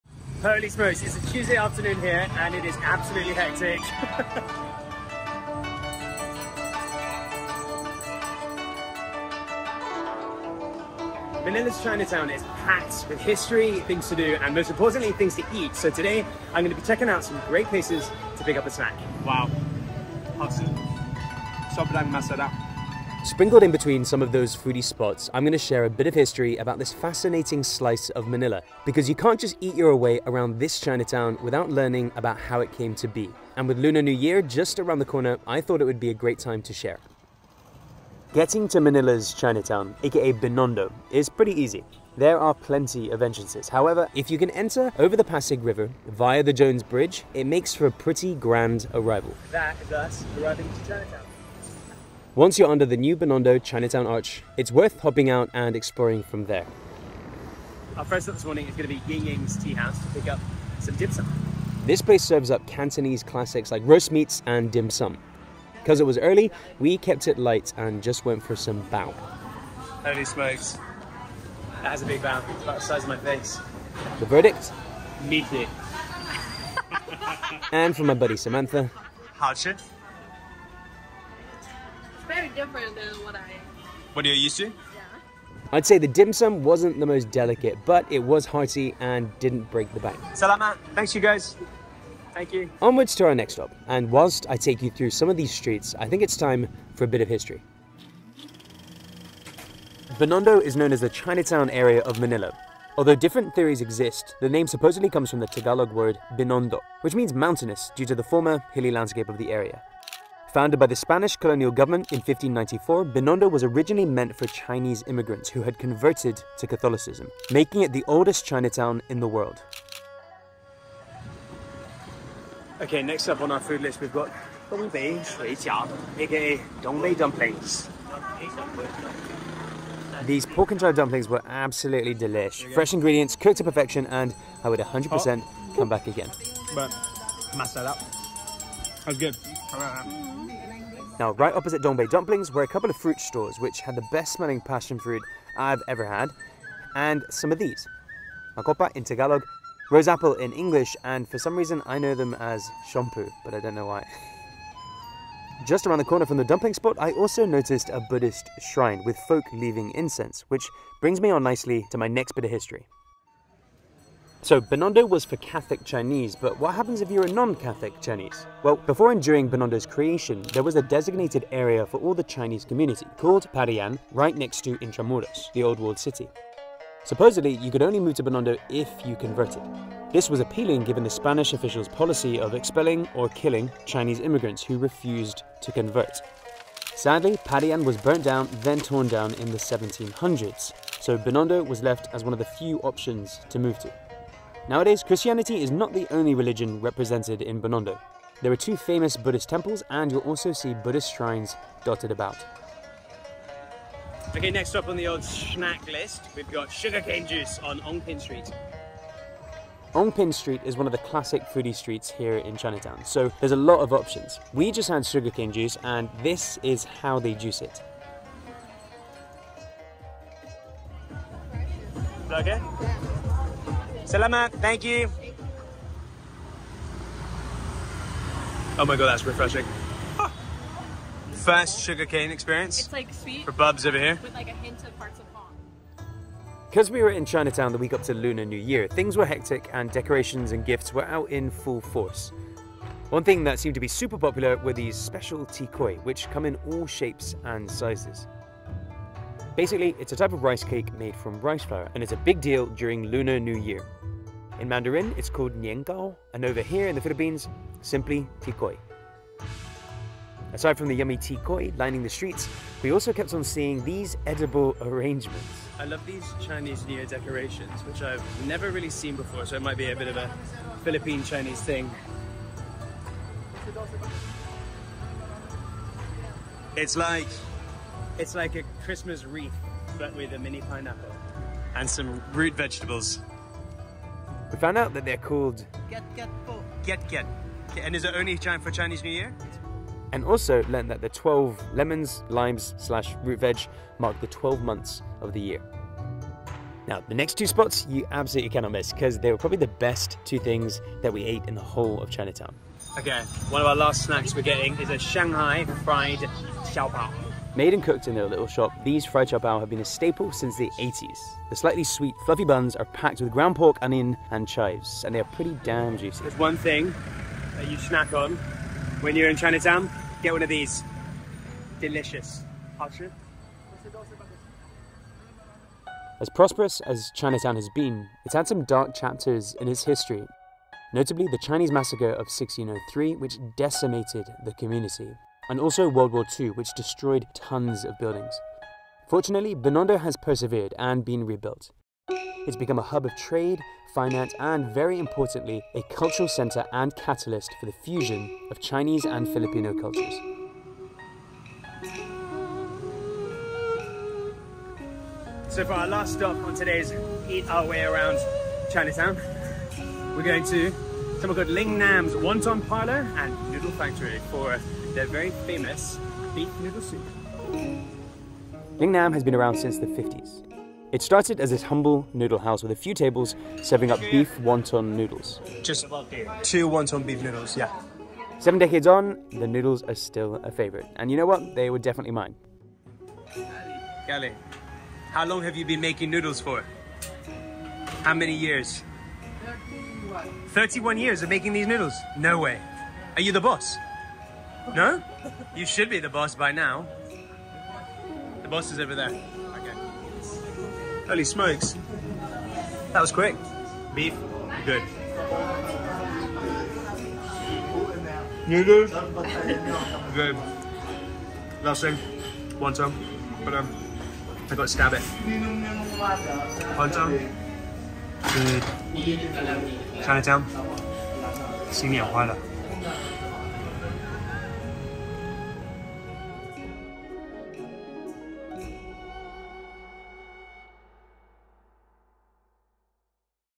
Trying MUST EAT foods in the World's Oldest Chinatown!
Today I'm embarking on a tasty journey through Binondo Chinatown, during the lead up to Lunar New Year. Join me on this mouthwatering tour as I explore some great places to grab snack in the heart of Manila's Chinese-Filipino culinary scene. From savoury dumplings to sugarcane beverages, hear about some popular foodie spots whilst I share the fascinating history of the oldest Chinatown in the world.

Find me here:
Main YouTube Channel:  @FirstPiper